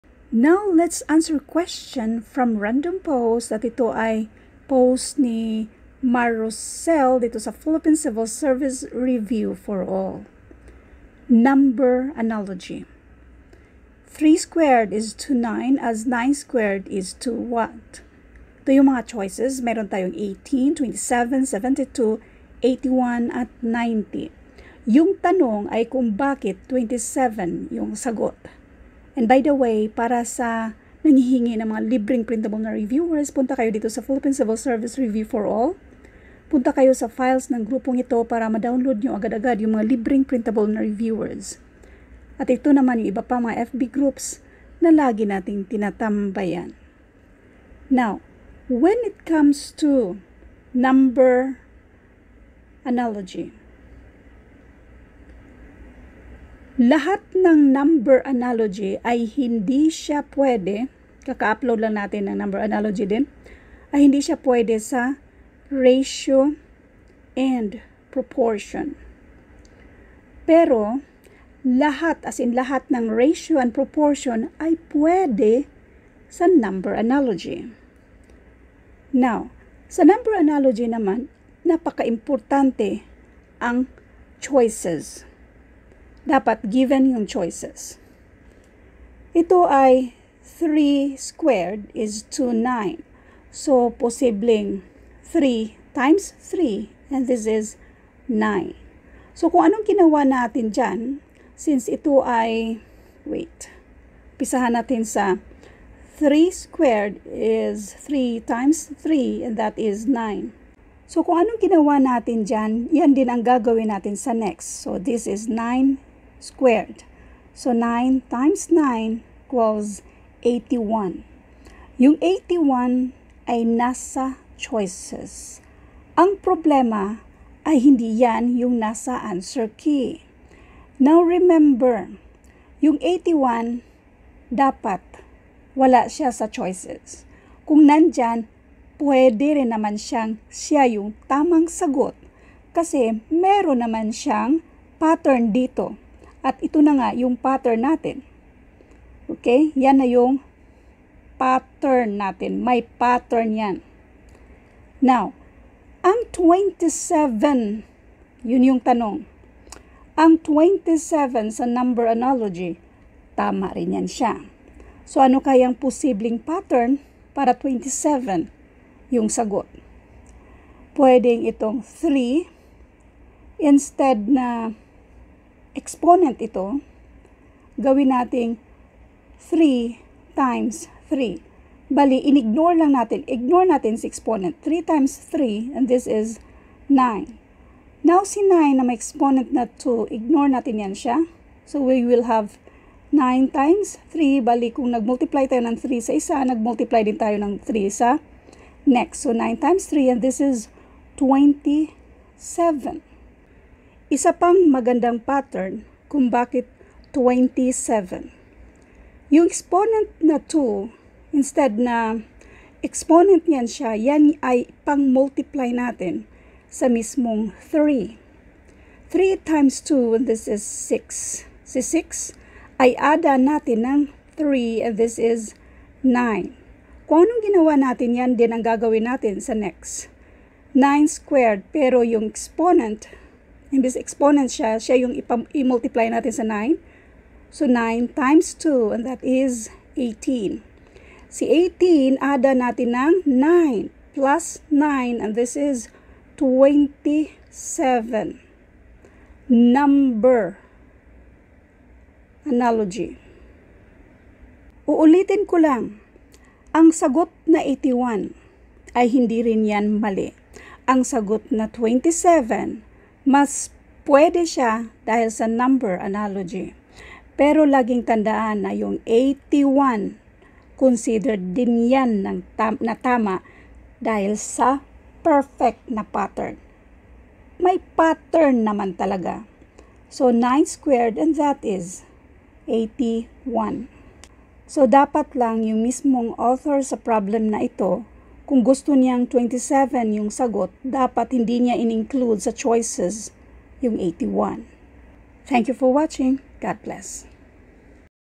NUMBER ANALOGY: 3² is to 9 as 9² is to ?
3² is to 9 as 9² is to _____?
a. 18
b. 27
c. 72
d. 81
e. 90

Free Civil Service Reviewers managed by Leonalyn

Philippine Civil Service Review for all

Editing Apps: VivaVideo
SamsungNote5: Snote
Back-up: SamsungNote9